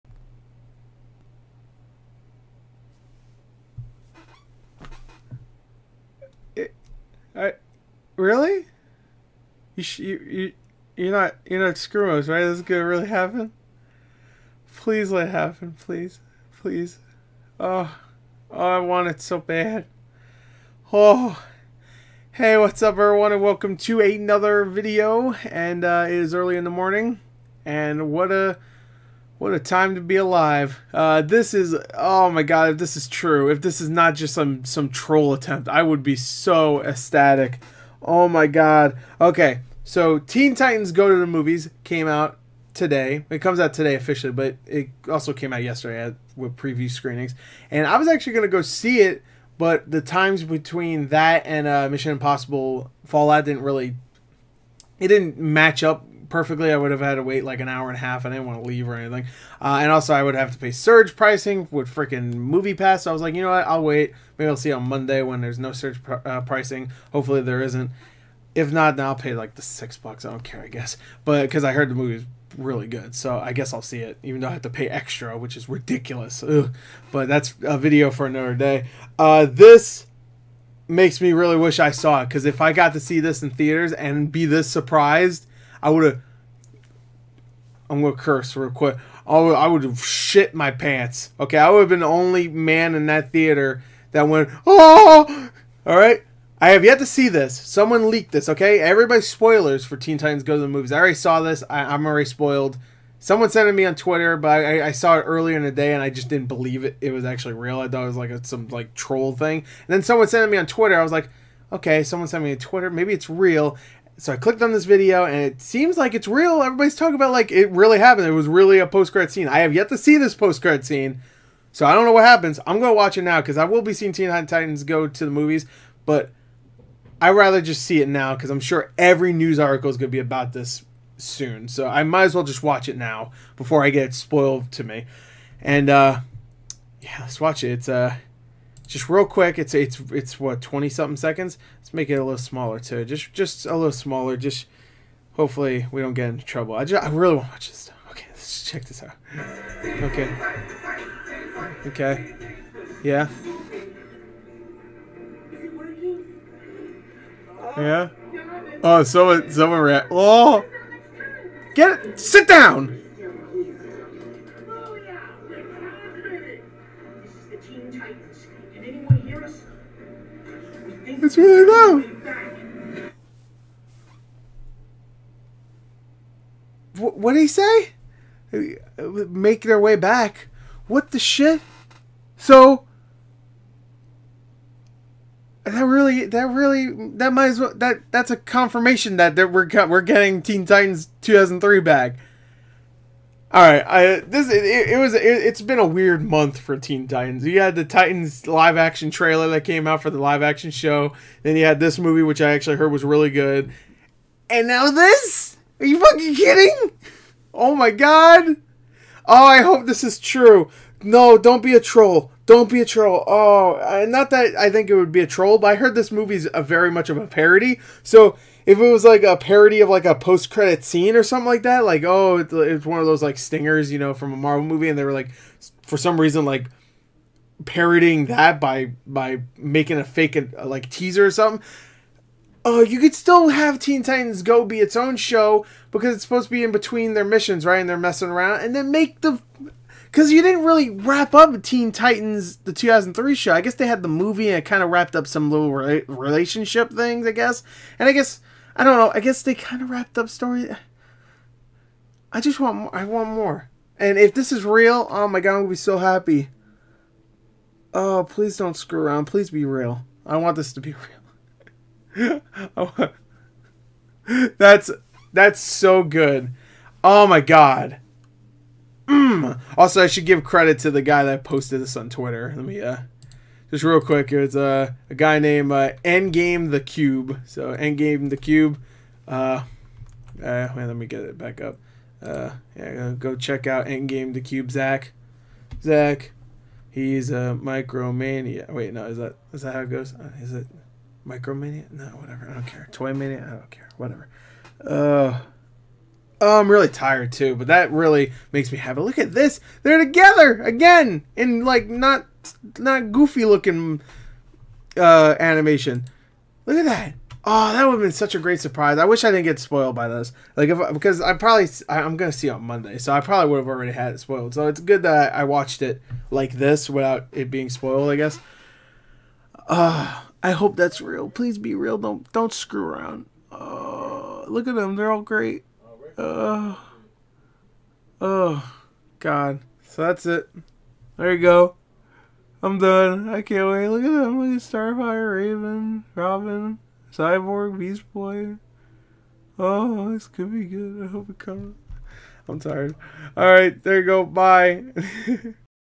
Original TEEN TITANS is Coming Back!?! - Gor's Thoughts
Teen Titans GO! to the Movies post credit scene teases a possible Teen Titans (2003) revival and Gor is a very excited little boy.